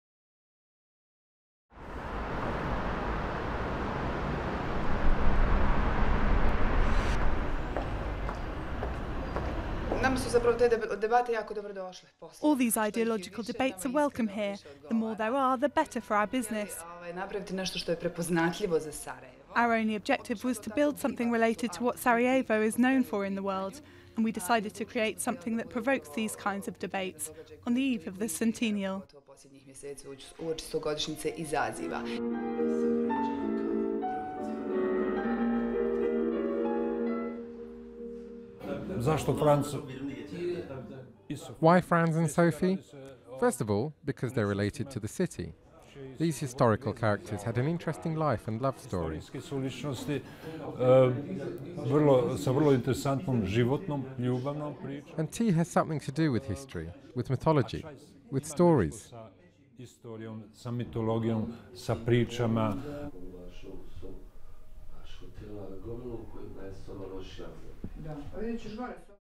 Century on, Franz Ferdinand and Sophie still 'live' in Sarajevo
To celebrate the national holiday of Vidovdan, Serbians remember the final moments in the life of Franz Ferdinand of Habsburg, heir to the Austrian-Hungarian Empire. His assassination in 1914 triggered the First World War. Duration: 01:09